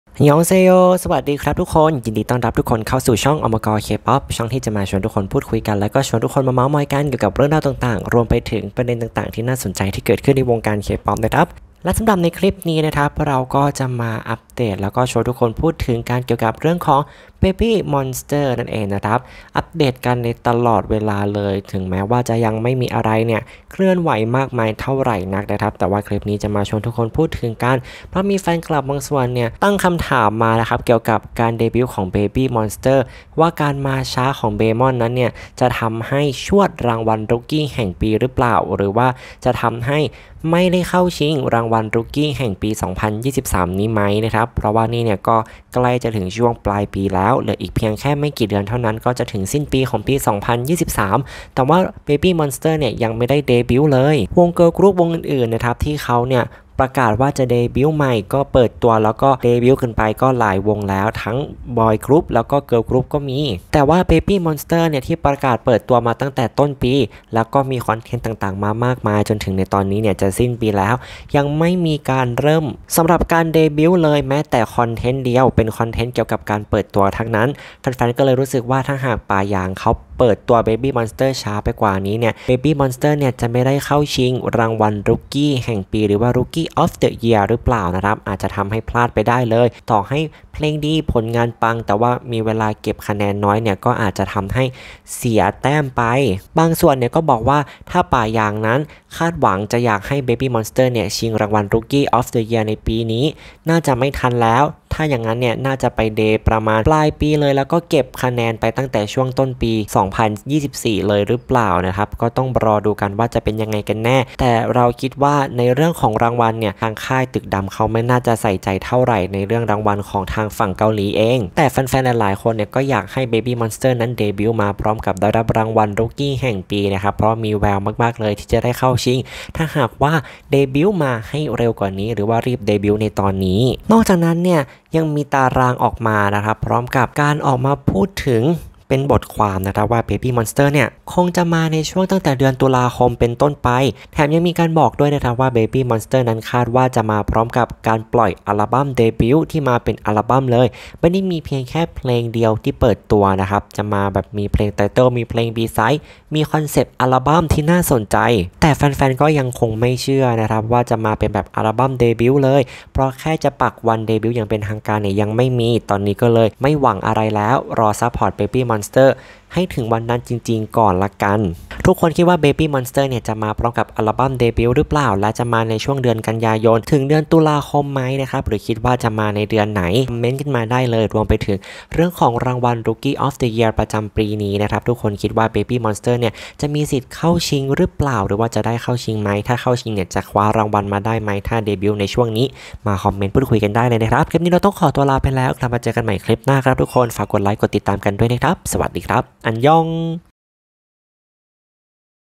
BABYMONSTER เดบิวต์ช้าเกินไปมั้ย จะทันชิงรุกกี้แห่งปีรึเปล่า มีสปอยล์งานนี้มาเป็นอัลบั้มจุกๆ
✨BABYMONSTER Official